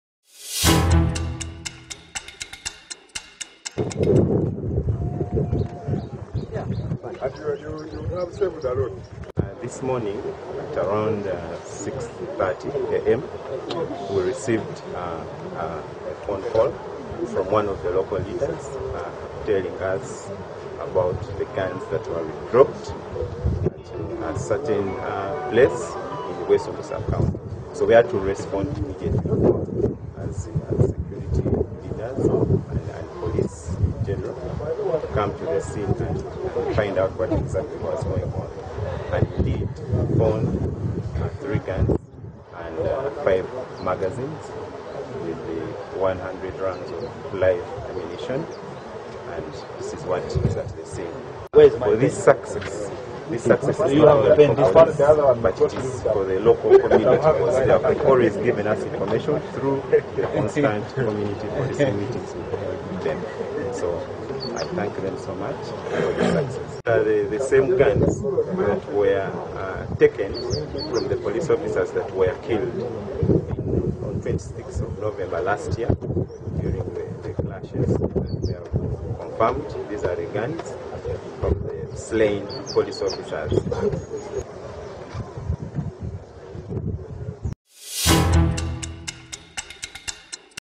NEW VISION TV: Guns lost during Kasese clashes recovered
The guns which were lost during the clashes when the Obusinga royal guards engaged the security in the area have been recovered. They had been hidden at a clinic